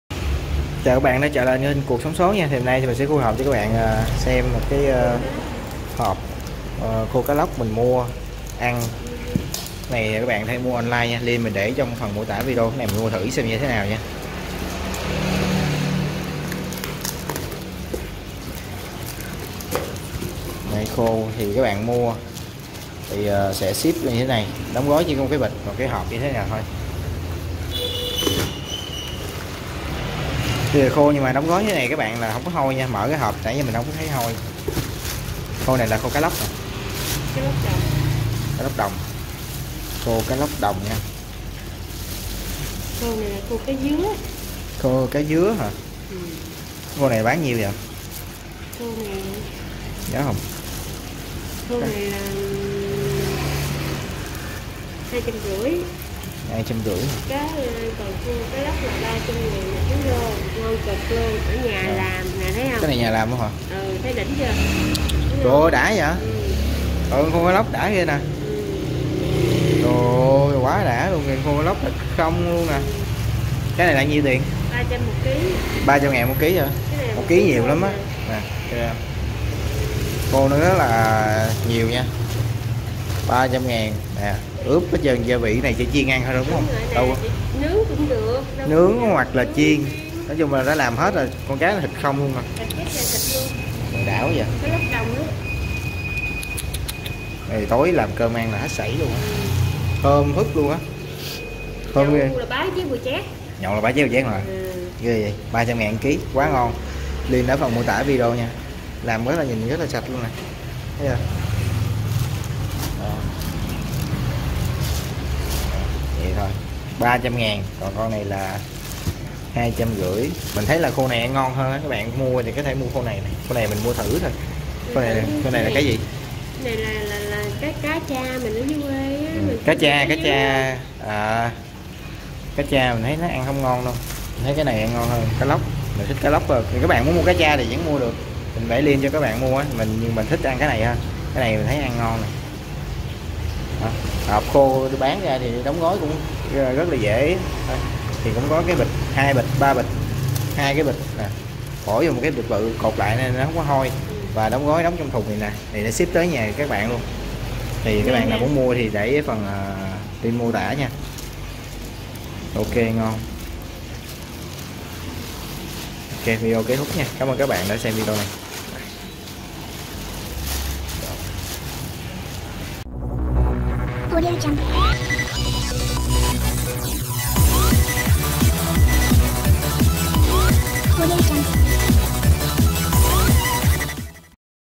Mua Thử Khô Cá Lóc Đồng Nhà Má Làm Online xem chất lượng thế nào 📺 Cuộc Sống Số 📺
►Create & Edit By Vũ Đình Phong (dinhphongtv)
+++ Video Liên Quan Khác: